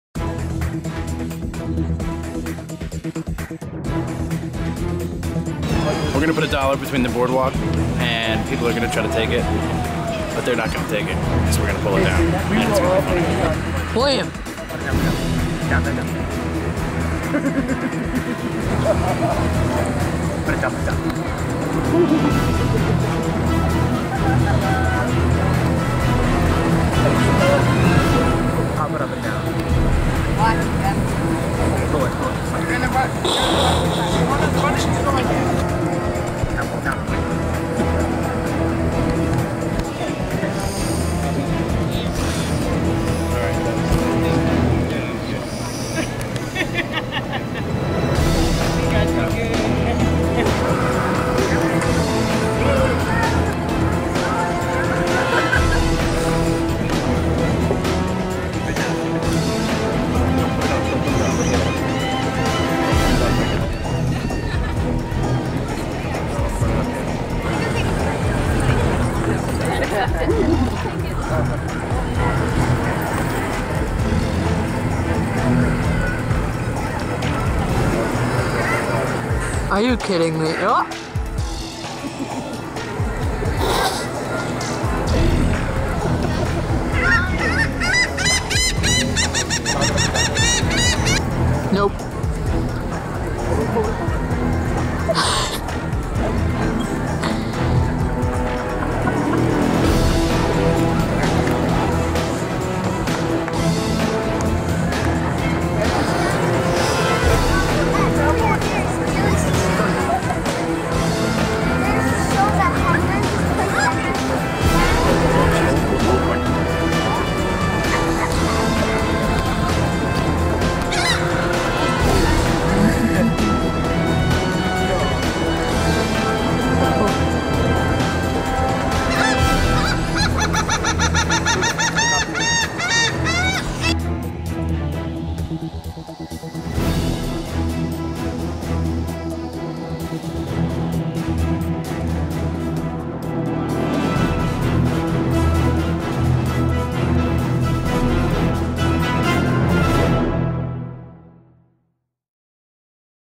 Boardwalk Dollar Prank 2
This is the second installment of making perfectly intelligent people feel like complete idiots.